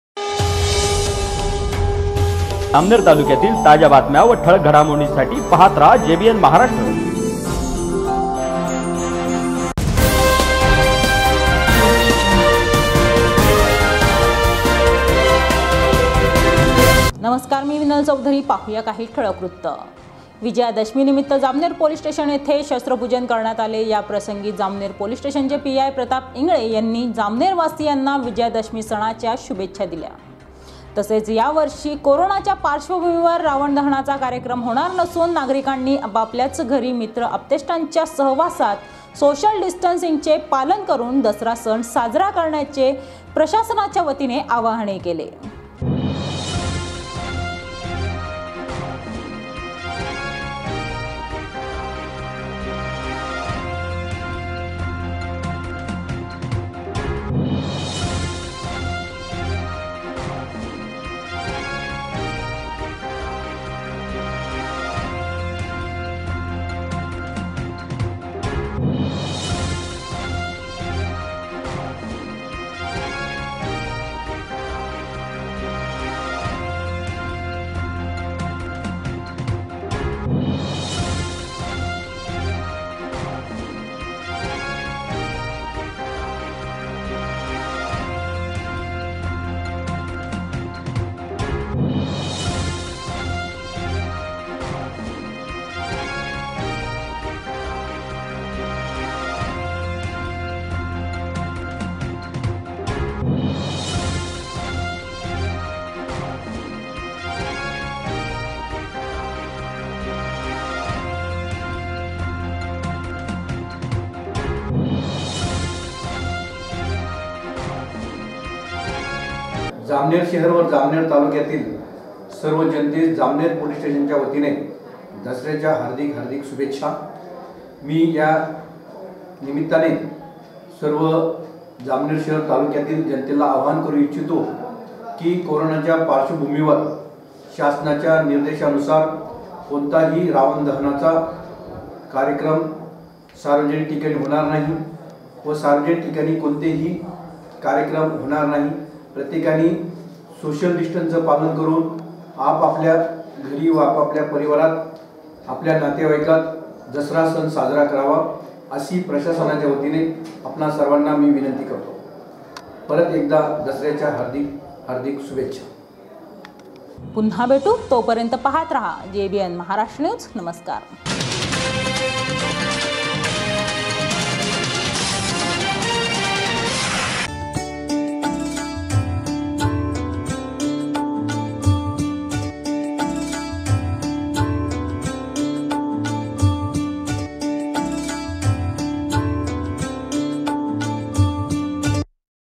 jbn maharashtra news jalgaon
विजयादशमी निमित्त  पोलीस स्टेशन येथे शस्त्रपूजन करण्यात आले.यावर्षी करुणा मुळे  रावण दहनाचा कार्यक्रम रद्द करण्यात आला .